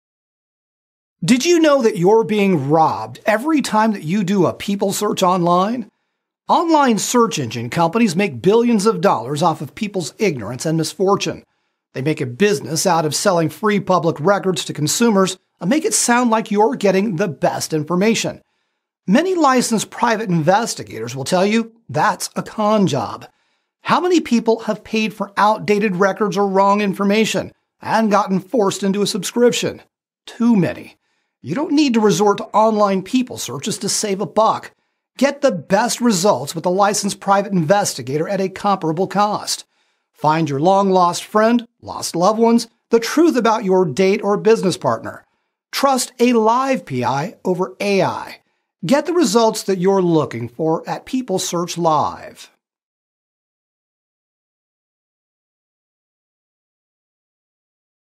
How The Online People Search Engines Scam You Update
Don’t get scammed online, trust a Live PI.

With PeopleSearchLive, you’re not getting just a search, you’re getting an investigation. A licensed private investigator conducts your search. Your Live PI searches deeper than public records. You’ll get accurate, real results. There are no subscription fees. All services are fully confidential. If nothing is found, you’ll receive a refund for the investigation fee.

3 Reasons Why You Should Not Use Online People Search Engines:
REASON 3
They don’t really give out any information for free, but force you to buy a subscription. Once you make a payment, you have a recurring subscription, which is time-consuming to cancel. You’ll end up wasting time and money.
REASON 2
They promise instant results, but what you get is mostly outdated data. Search results are only as good as the source, i.e. the databases. Some online databases are not legit. Many databases contain inaccuracies. They’re also incomplete. You’re wasting time and won’t be able to take immediate action.
REASON 1
They give you general information but not good data, because they don’t have a license to access it. Only licensed professionals can access information normally available to law enforcement or legal entities. Only a licensed professional can give you current details.

Don’t waste time, if you want to find somebody. Have one of our Live PI’s find you good information you can act on with confidence. A real person is doing it, and the information is current and accurate.

Transcription:
Did you know you’re being robbed every time you do a people search online? Online search engine companies make billions of dollars off of people’s ignorance and misfortune. They make a business out of selling free public records to consumers and make it sound like you’re getting the best information. Many licensed private investigators will tell you that’s a con job.

How many people have paid for outdated records or wrong information? And gotten forced into a subscription? Too many! You don’t need to resort to online people searches to save a buck. Get the best results with a licensed private investigator at a comparable cost.

Find your long lost friend, lost loved ones, or someone who owes you money. Find out the truth about your date or business partner. Trust a Live PI over AI.

PeopleSearchLive is powered by Courthouse Investigative Agency (CIA), a licensed private investigative agency.